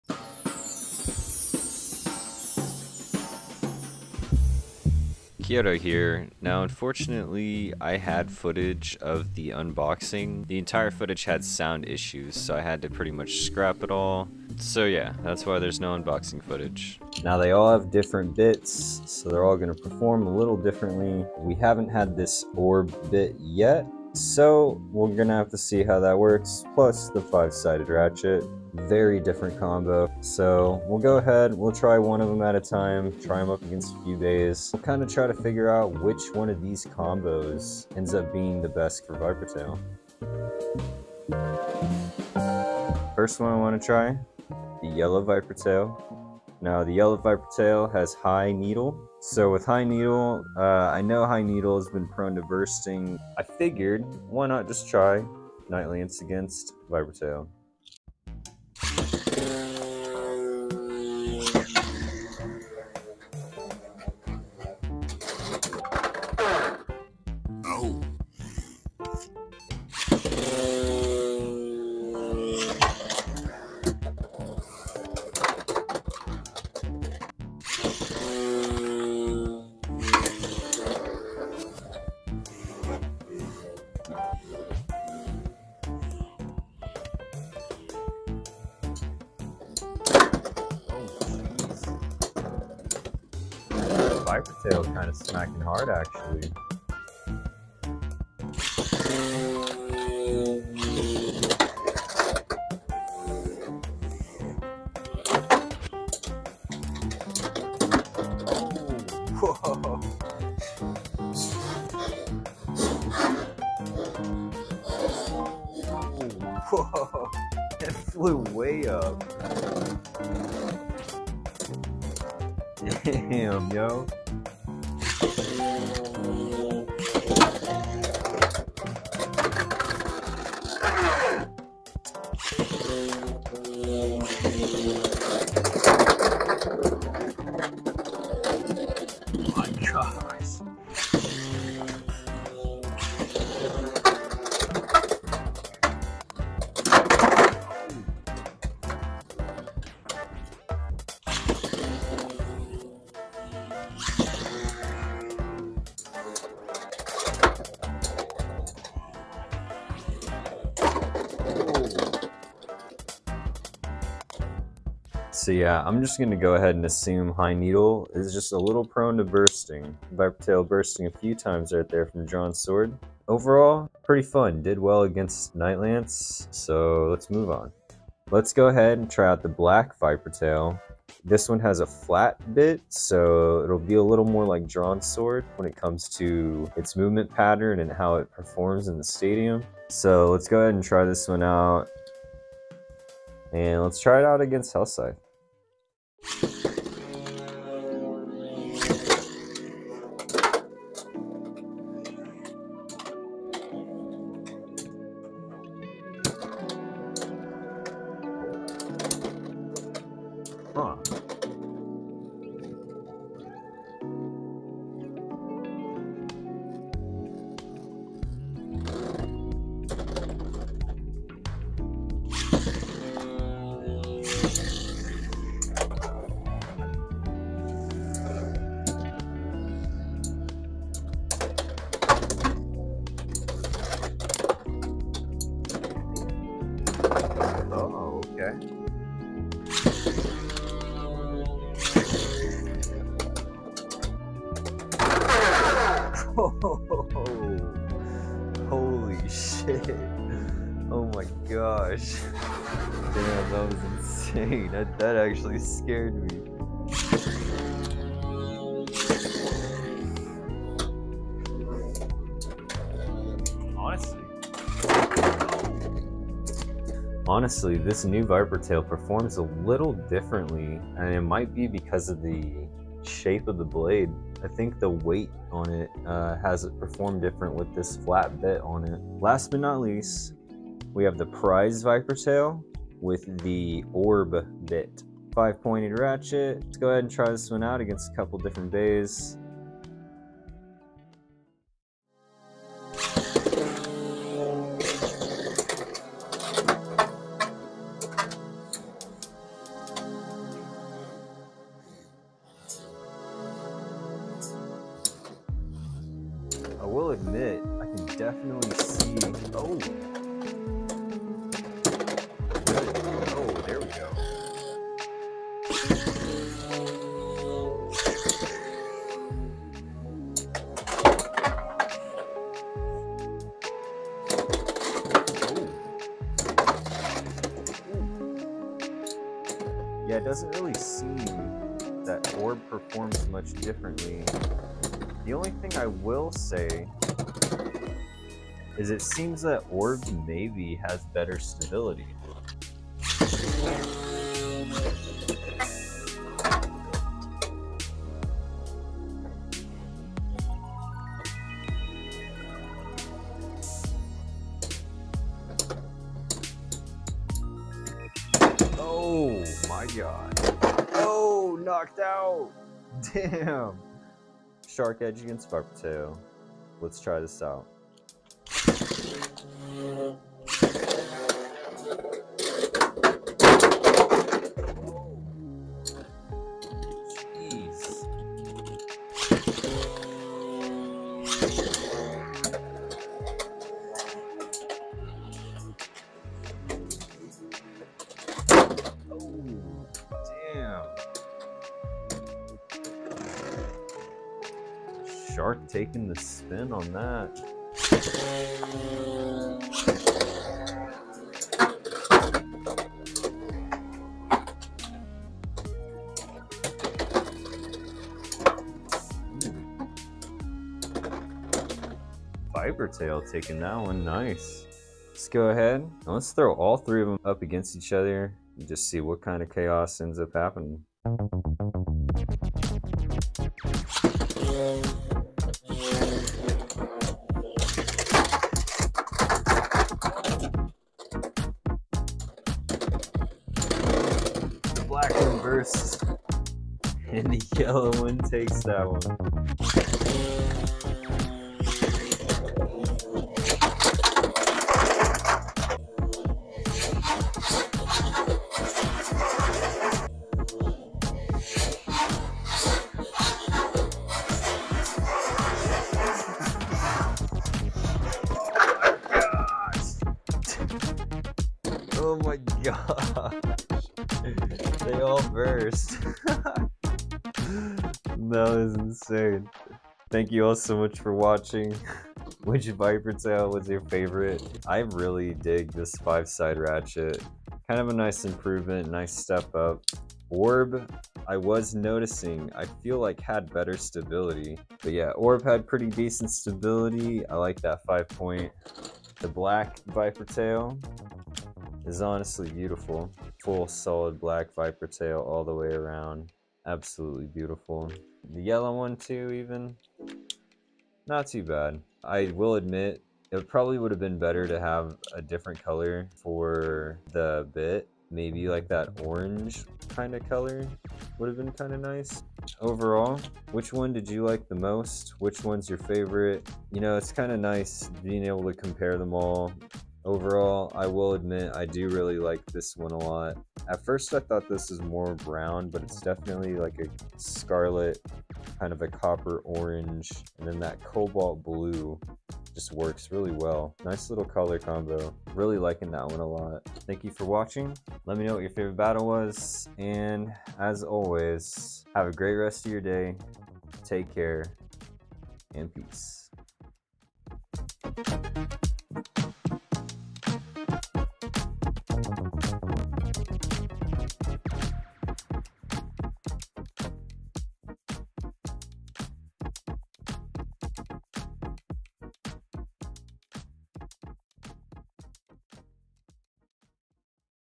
*NEW* VIPER TAIL is ABSOLUTELY INSANE! Testing Out All The Viper Tail Beyblades. ( BEYBLADE X )
IT CONTAINS INNAPROPRIATE LANGUAGE and/or
THEMES NOT SUITABLE FOR KIDS!

This Video is INTENDED For An AUDIENCE of COLLECTORS,
HOBBYISTS, ENTHUSIASTS, and COMPETITIVE PLAYERS! )

The NEW VIPER TAIL is ABSOLUTELY INSANE!

Today we will be trying out all (3) of the NEW VIPER TAIL Combos to see which one is the BEST!

Thank you and have an amazing day!

Peace!